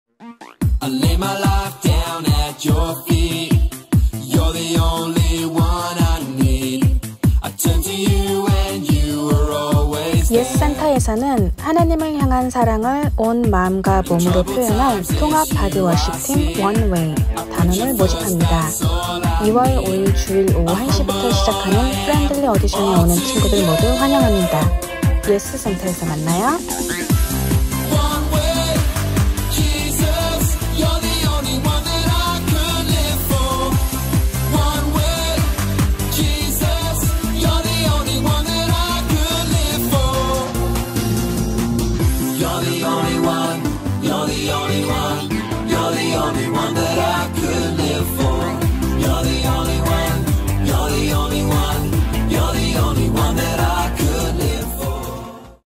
YES Body Worship Team | One Way (한국어)
YES( Young Nak Education Support ) Center Body Worship Team ‘One Way’
하나님을 향한 사랑을 온 마음과 몸으로 표현할 통합 바디워십팀 ‘OneWay’ 단원을 모집합니다!
2월 5일 주일 오후 1시부터 시작하는 Friendly Audition에 오는 친구들 모두 환영합니다!